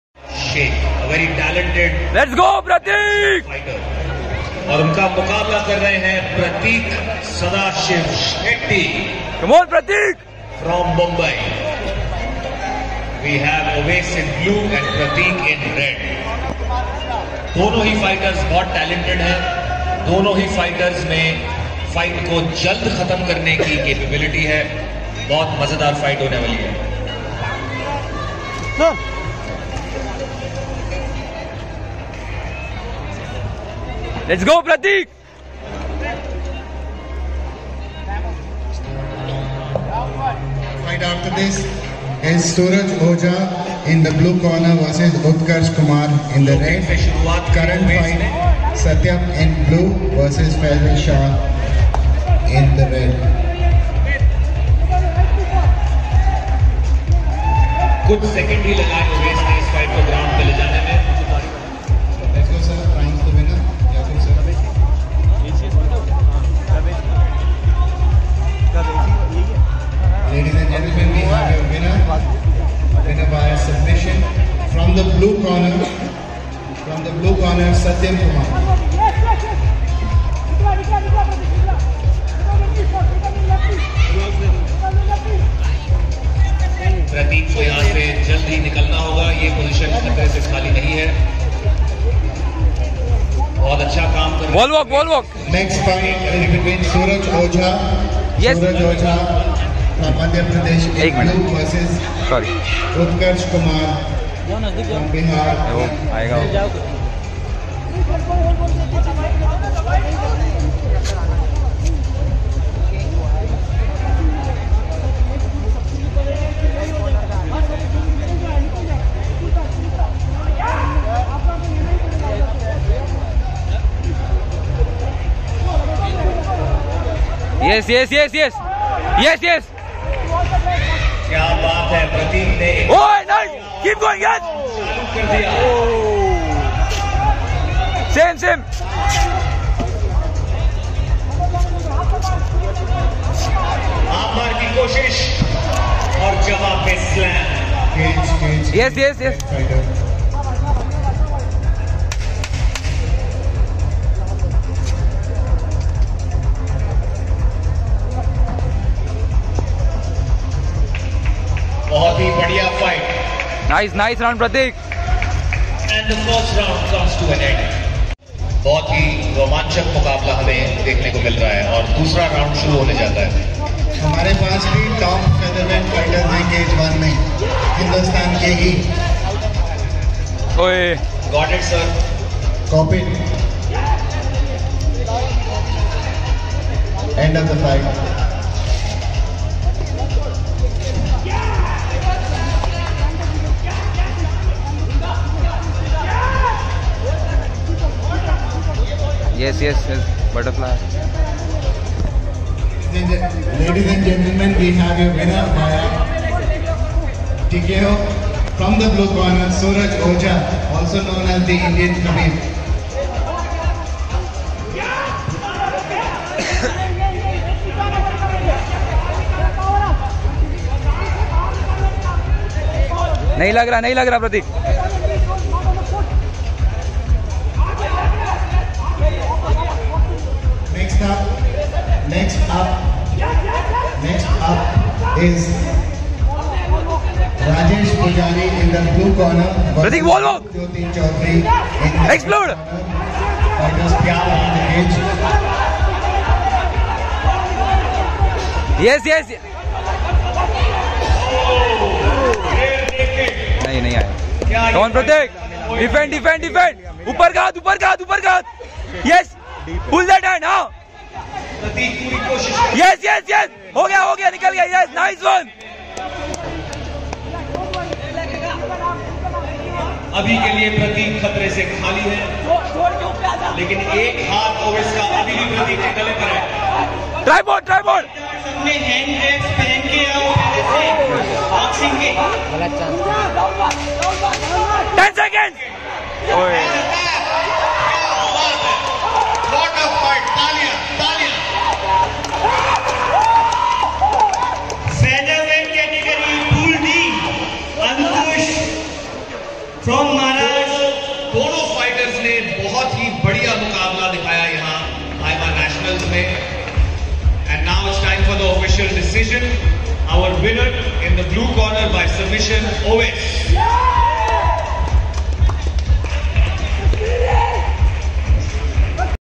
OWAIS YAQOOB VS PRATIK SHETTY BEST MMA FIGHT EVER
This fight was the best among 400 fights held at Indoor stadium Hyderabad on 30th of September 2022. Ovias yaqoob won the fight against an undefeated and surely the best grappler Prateek Shetty by a powerful RNC (rare naked choke) in second round.